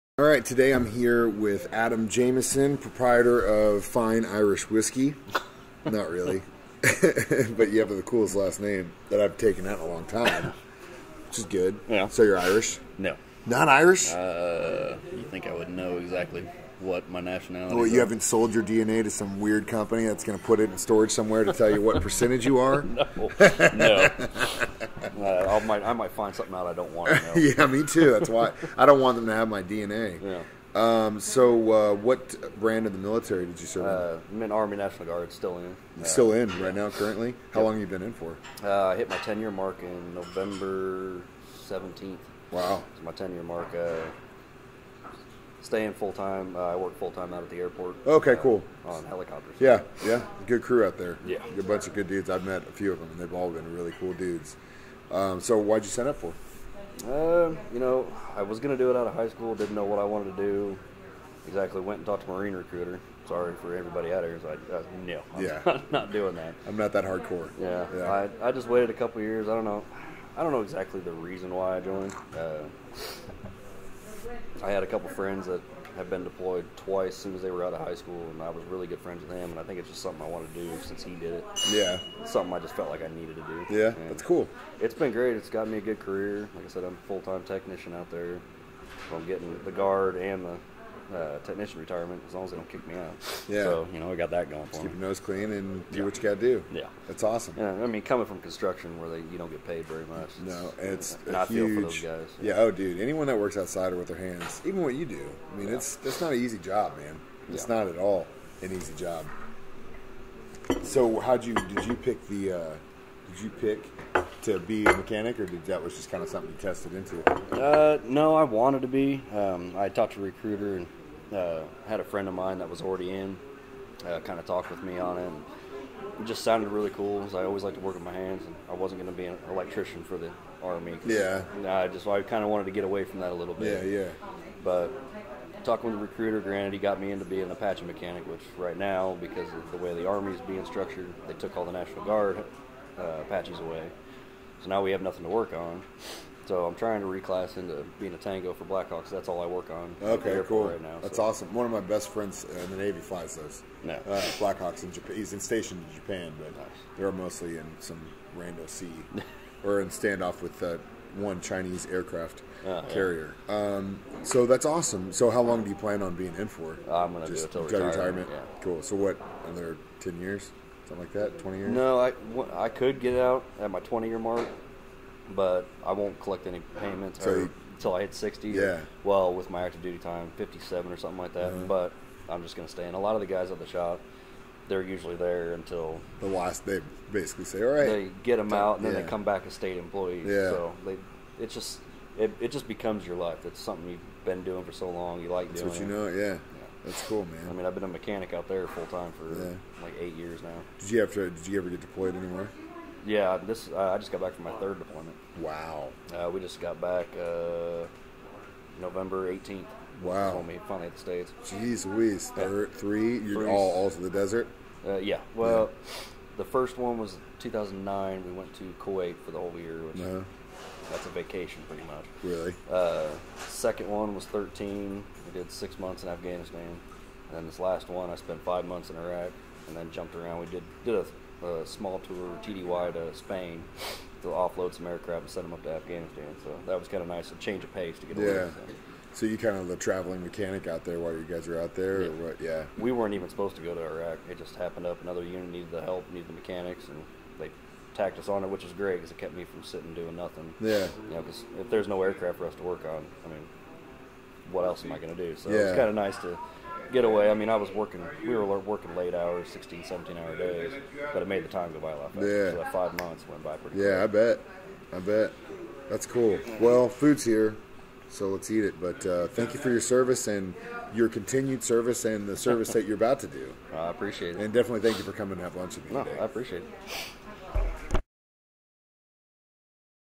LUNCH WITH A HERO ADAM JAMIESON 12.13.17
This is Adam! He currently serves in the National Guard and had lunch with me today! Hear his story tomorrow morning during my show at 8:45am. Thank you for your service!